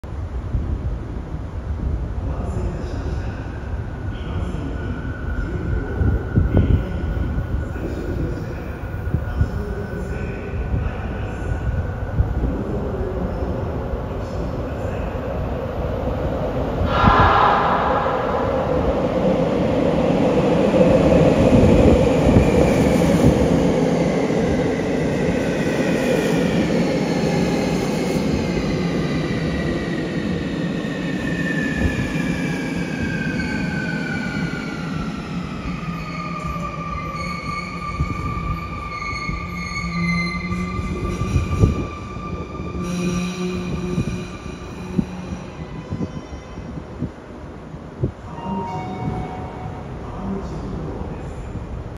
相鉄21000系 急行海老名行 川口元郷駅到着
車両:相鉄21000系電車
急行　海老名行き
埼玉高速鉄道 川口元郷駅
2023年3月撮影。

4K60fpsバージョンはこちらです。